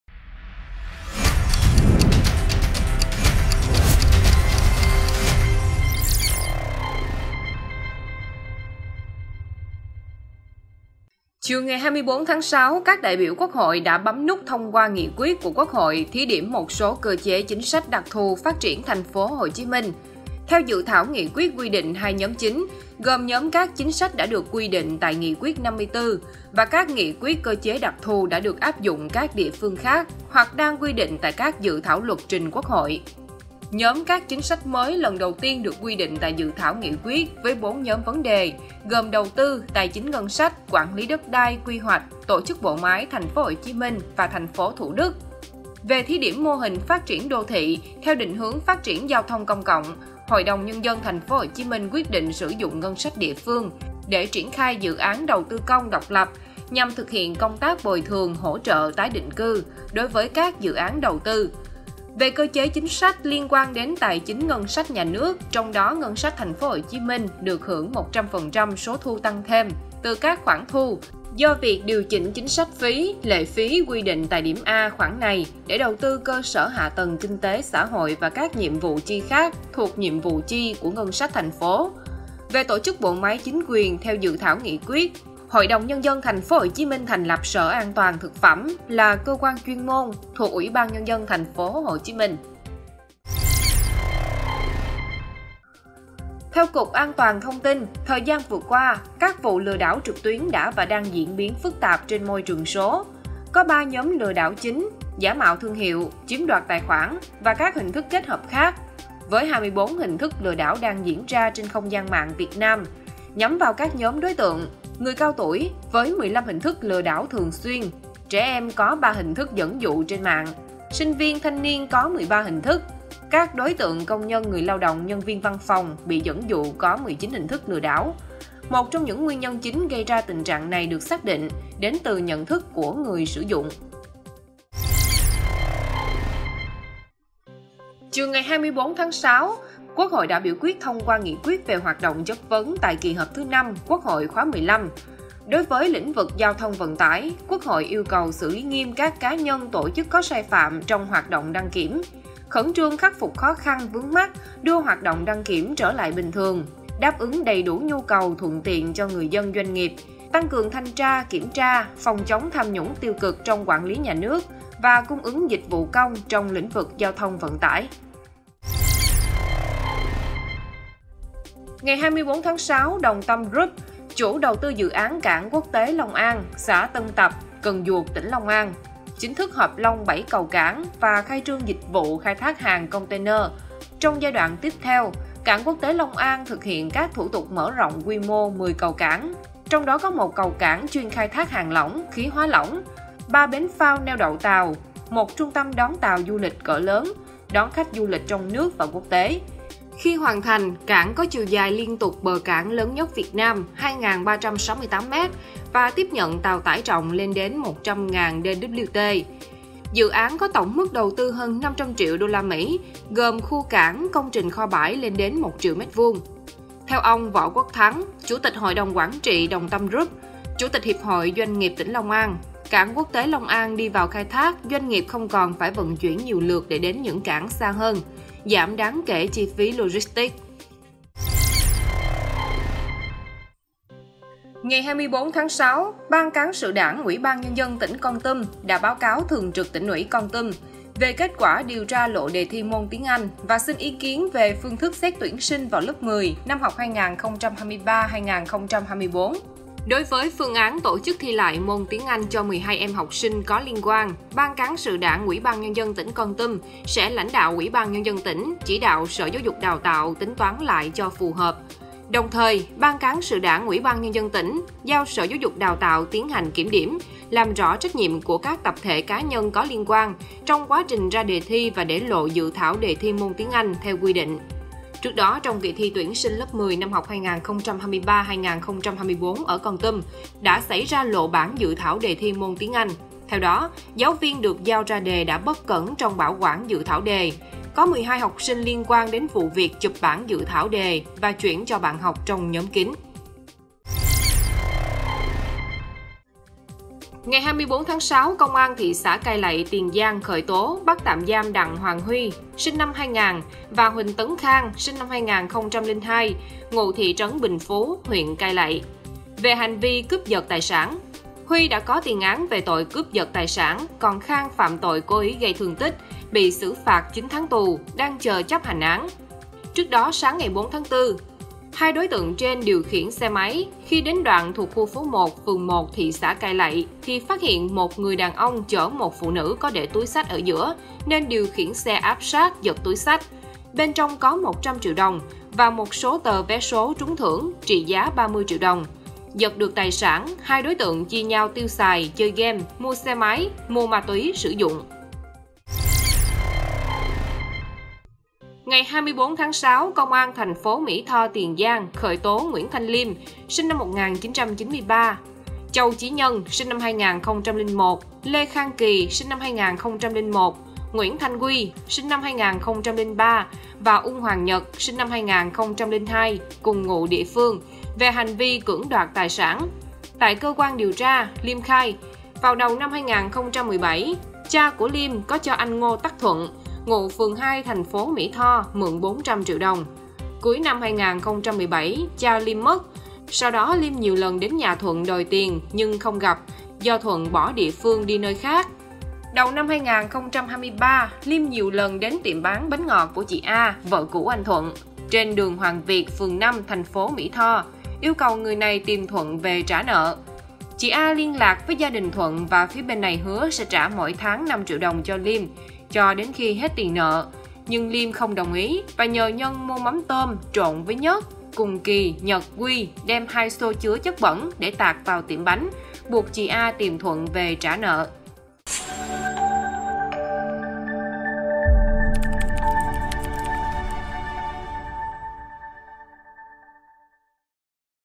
Cảng quốc tế Long An hợp long 7 cầu cảng; Khởi tố nhóm “khủng bố” tiệm bánh để đòi nợ | Tin nóng 20H
Tin nóng 20H ngày 24-6 có các thông tin đáng chú ý: Quốc hội thông qua Nghị quyết đặc thù phát triển TPHCM; Cảnh báo về 24 hình thức lừa đảo trên không gian mạng; Làm rõ trách nhiệm tổ chức, cá nhân để lộ dự thảo đề thi môn Tiếng Anh ở Kon Tum…